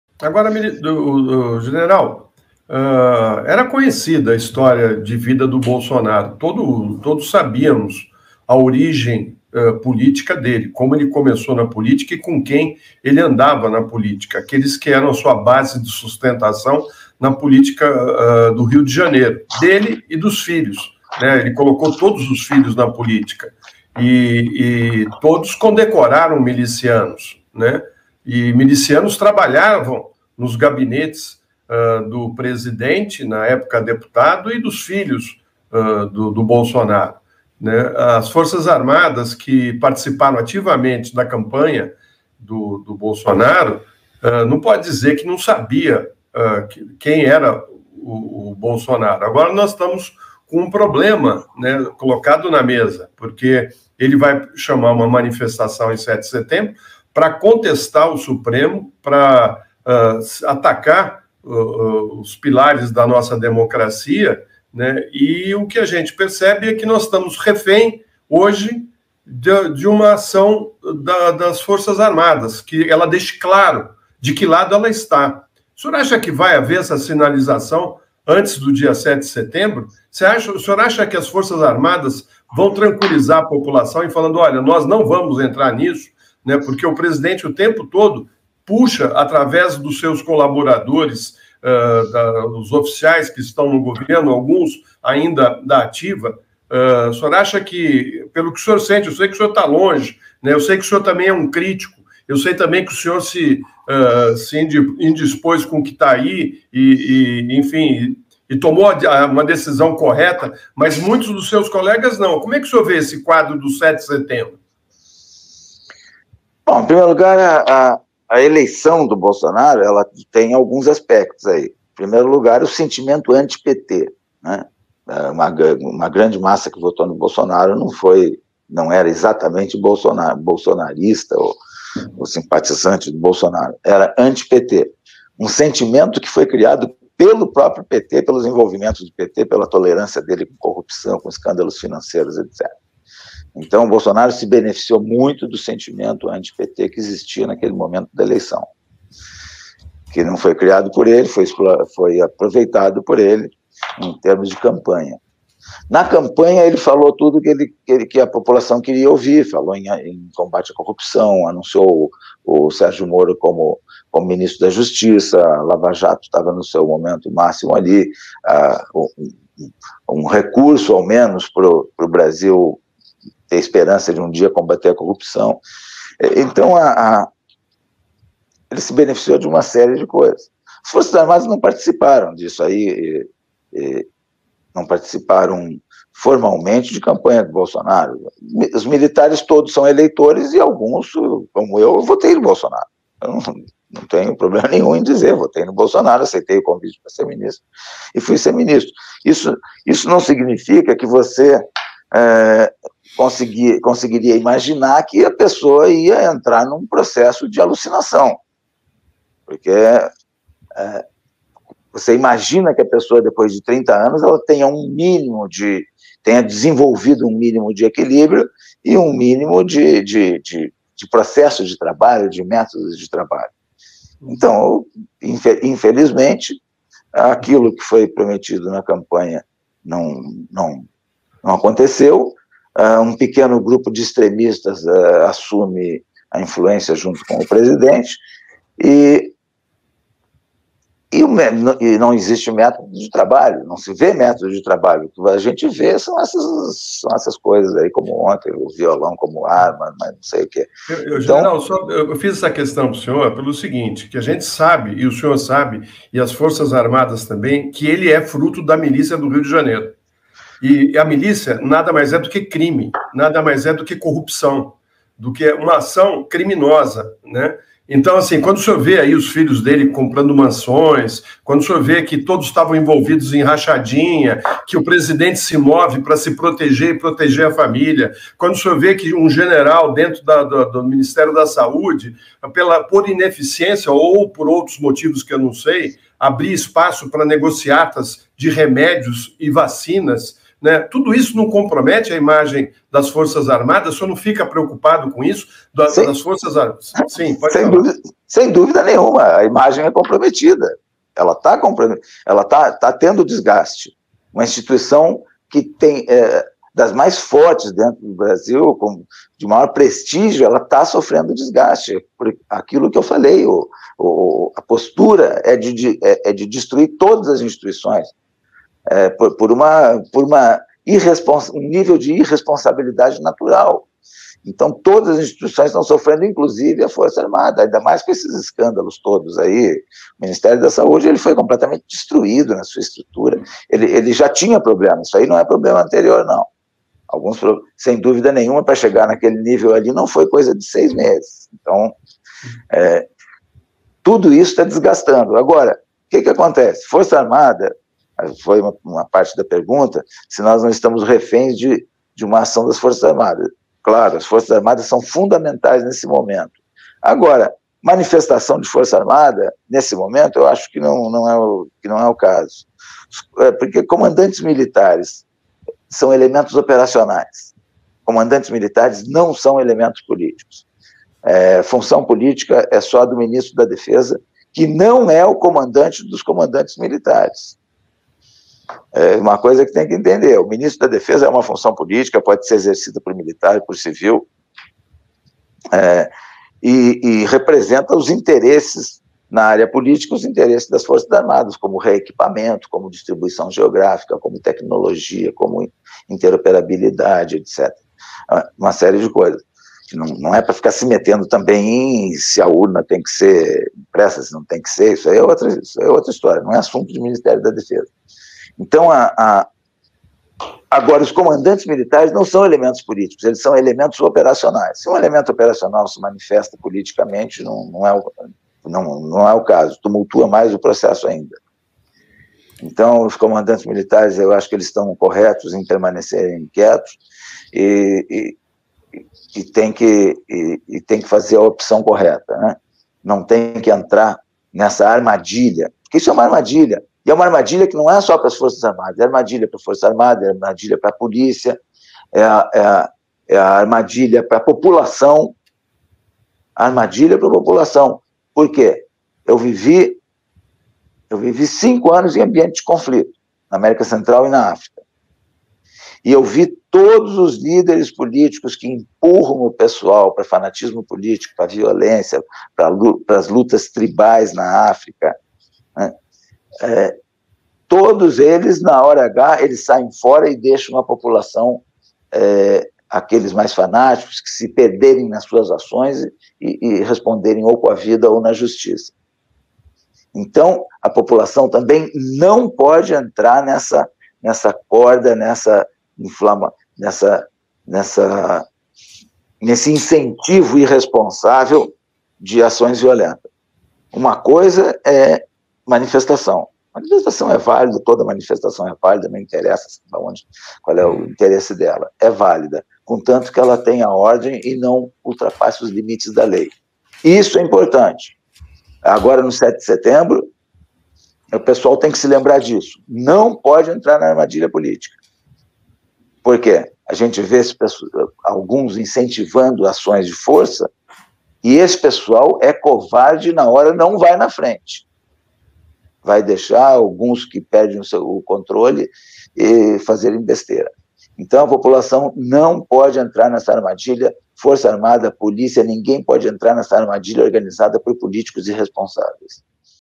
Santos Cruz diz que votou em Bolsonaro sem imaginar que ele entraria em um “processo de alucinação”
Em entrevista à TV Democracia nesta segunda-feira (30), o ex-ministro da Secretaria da Presidência, general Santos Cruz, comenta o apoio e o voto que concedeu a favor de Jair Bolsonaro (sem partido) nas eleições de 2018. O general afirma que, a despeito do voto e de ter virado ministro do Governo, “não conseguiria imaginar que a pessoa ia entrar em processo de alucinação”.

“Eu votei no Bolsonaro, não tenho nenhum problema em dizer. Votei em Bolsonaro, aceitei convite para ser ministro e virei ministro. Isso não significa que você conseguiria imaginar que a pessoa ia entrar em um processo de alucinação”, afirma o general.

Santos Cruz complementa afirmando que o presidente descumpriu promessas de campanha.

“Você imagina que depois de 30 anos, a pessoa tenha desenvolvido um mínimo de equilíbrio e de métodos de trabalho. Então, infelizmente, aquilo que foi prometido na campanha não aconteceu. Um pequeno grupo de extremistas assume a influência junto com com o presidente e não existe método de trabalho”, afirma.

💡 ACESSE JÁ!